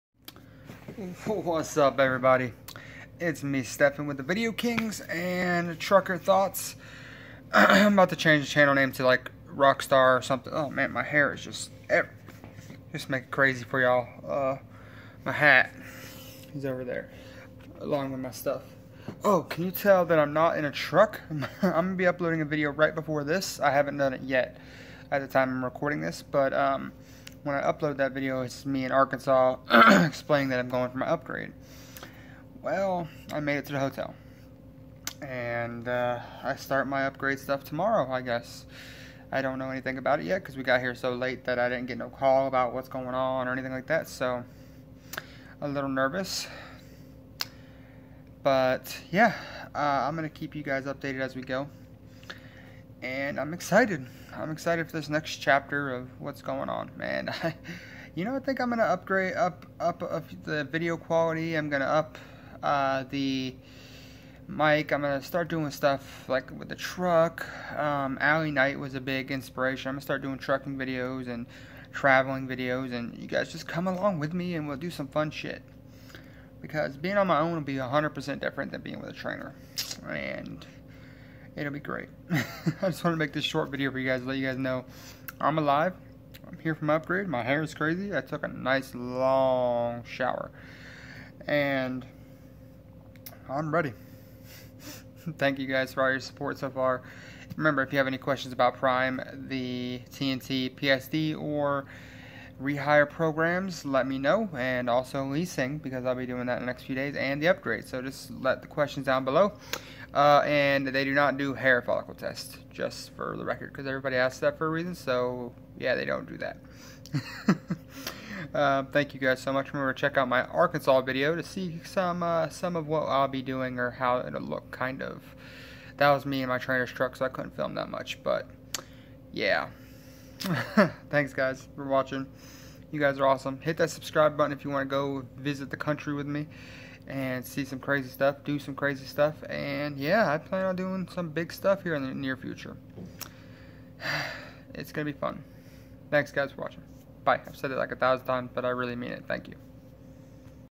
Prime Inc UPGRADE TIME
Just got to prime after 40000 miles tike to upgrade